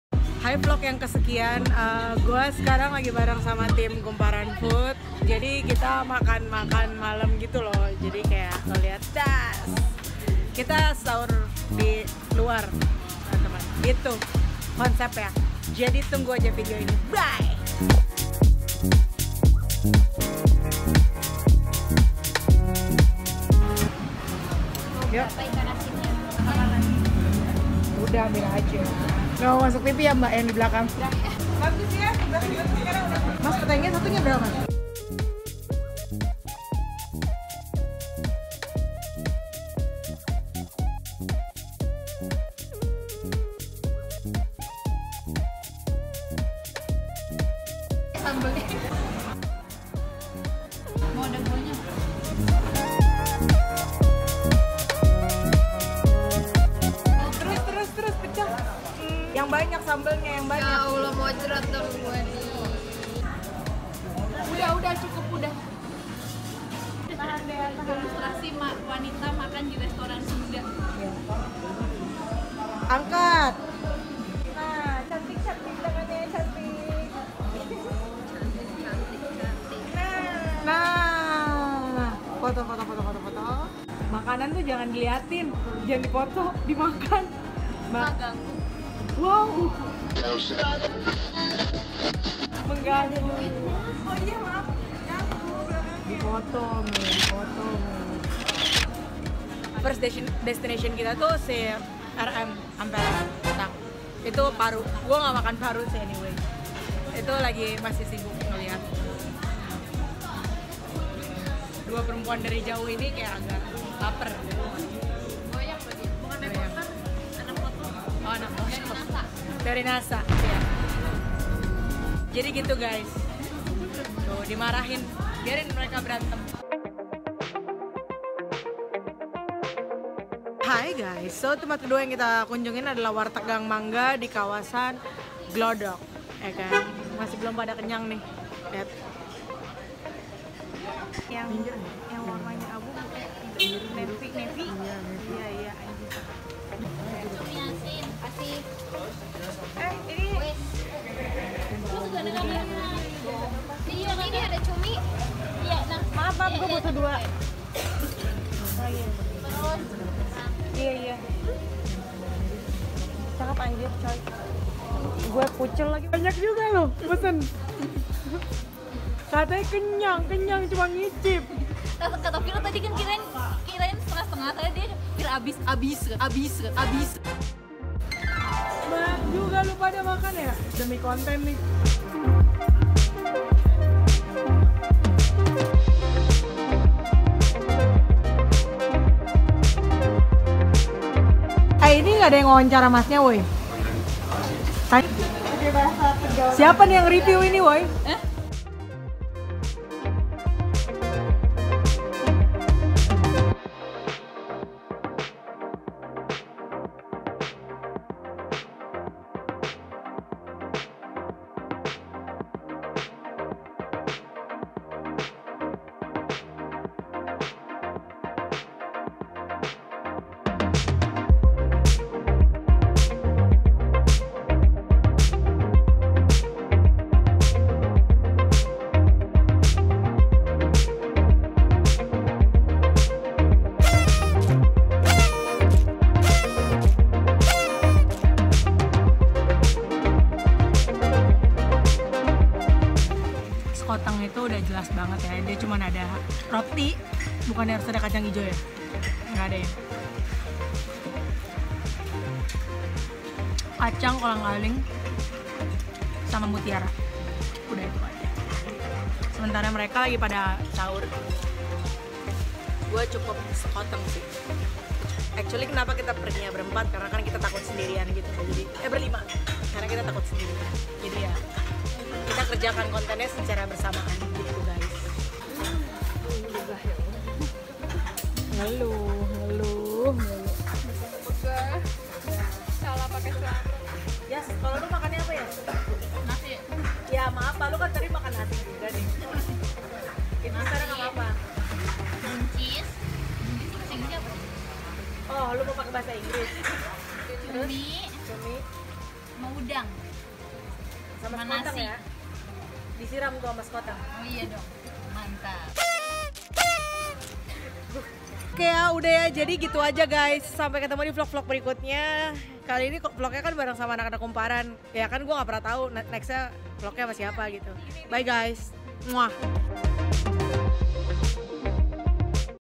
Makan Ini Itu #4: Makan 'Tengah Malam' Bareng | Warteg Gang Mangga dan Ampera 2 Tak
Gue bareng tim kumparanFOOD makan tengah malem nih. Review jelasnya, langsung ya ke bawah ini: